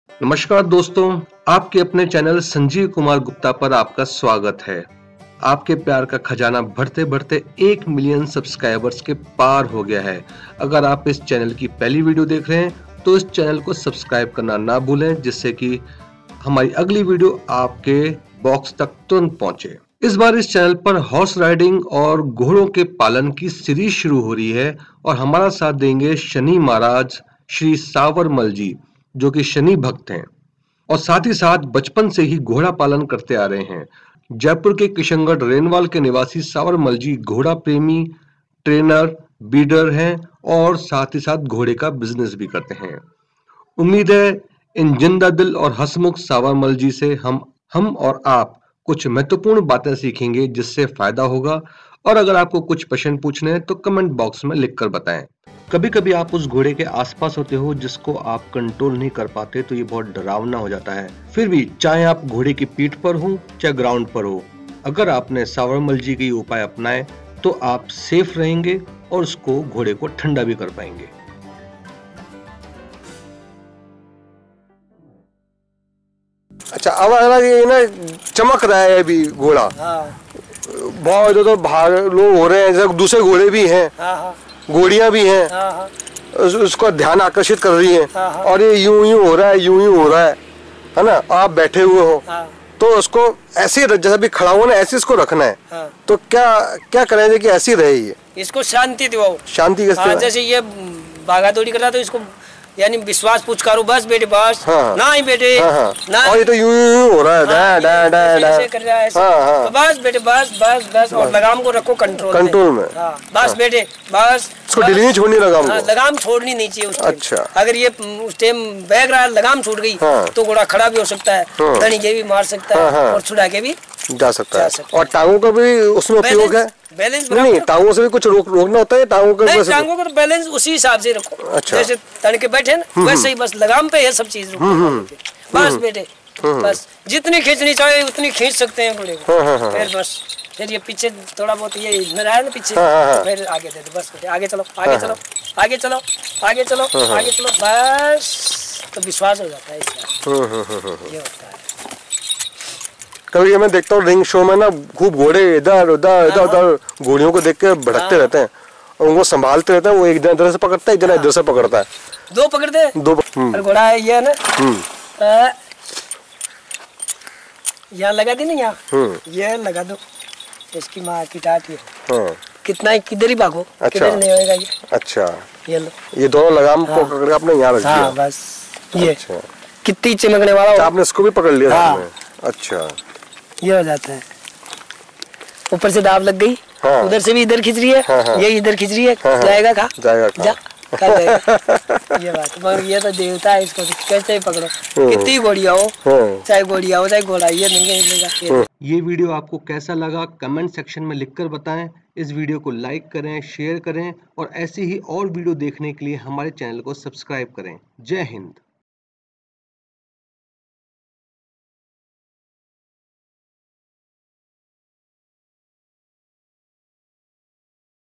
जान लो चमकते घोड़े को कैसे कण्ट्रोल करें How To Control Alarming Horse - Horse Riding Training
FSA ENTERTAINMENT FSA ENTERTAINMENT In this horse training video a very good horse trainer - Mr. SawarMal teaches a basic horse training of horse riding lesson  to learn how to ride a horse .
इस विडियो में घुड़सवारी कैसे करें उसका बुनियादी पाठ पढाया गया है .
चमकते घोड़े को कैसे कण्ट्रोल करें  How To Control Alarming Horse

Tutorial By horse trainer - SawarMal Shani Maharaj, Kishangarh Renwal, Jaipur, Rajasthan

Horse Video घोड़ा विडियो